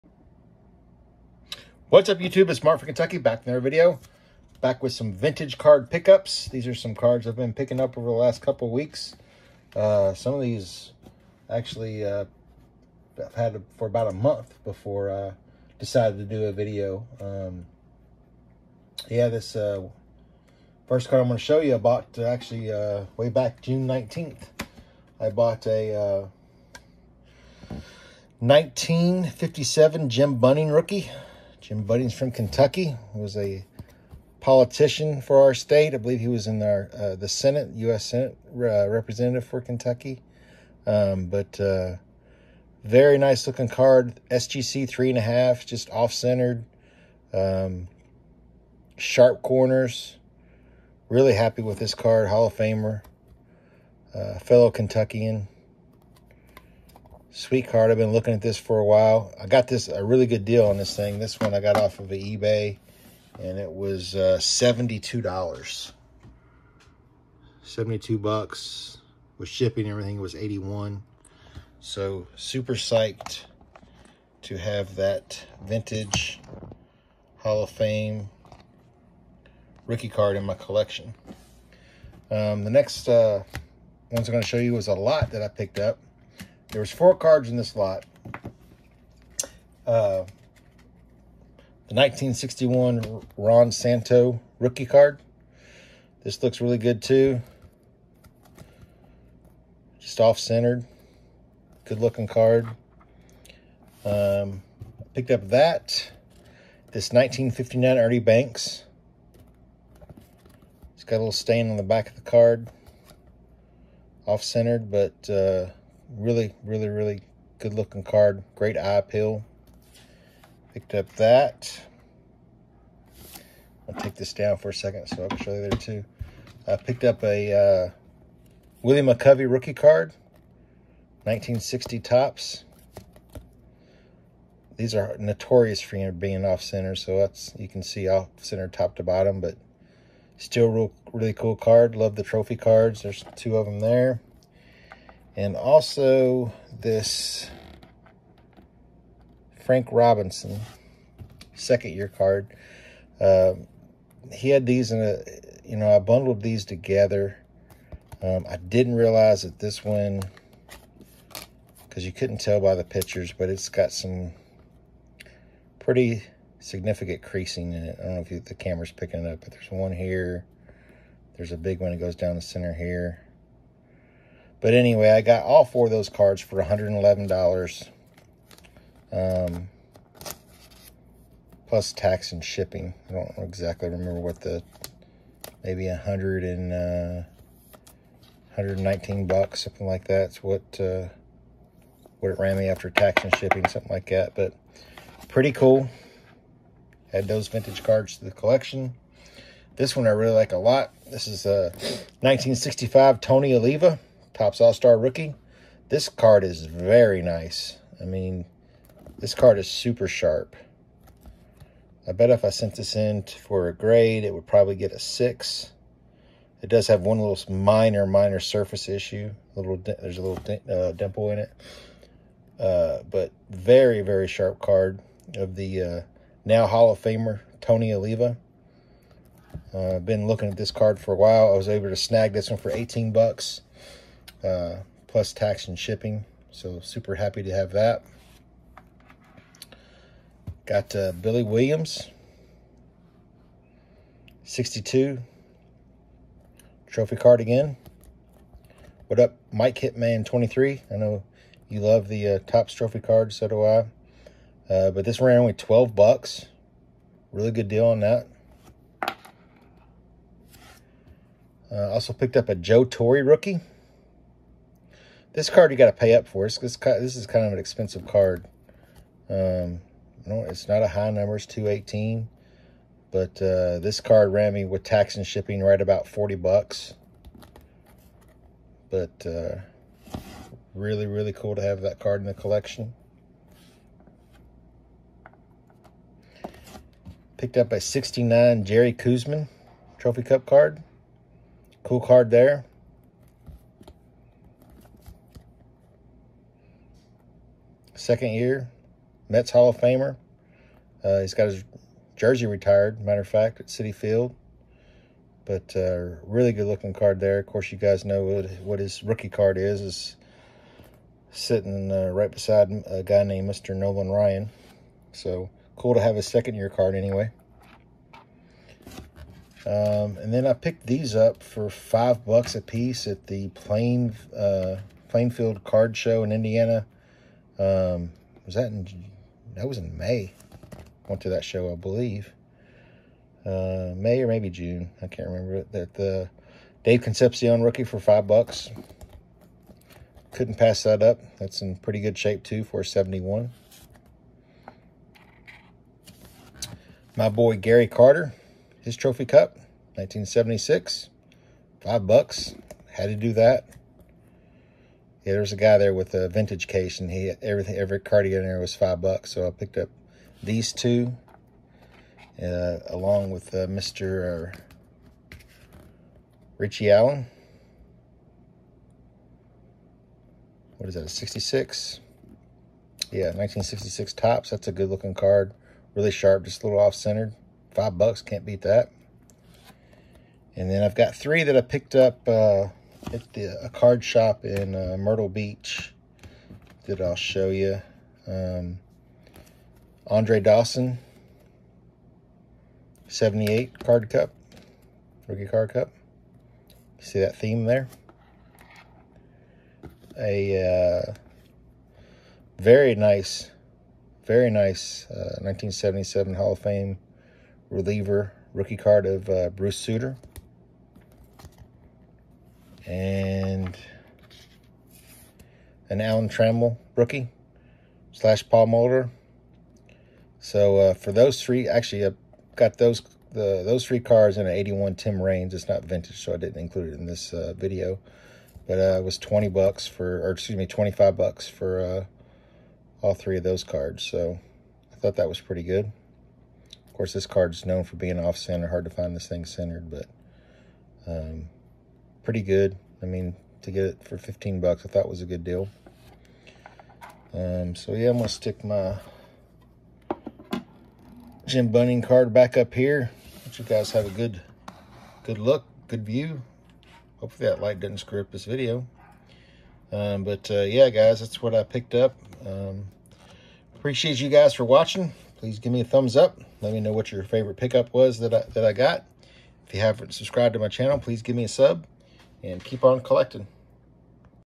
Vintage Vintage Vintage!!!!!
Picked up some 1950’60’s70’s Vintage cards and vintage Topps trophy cards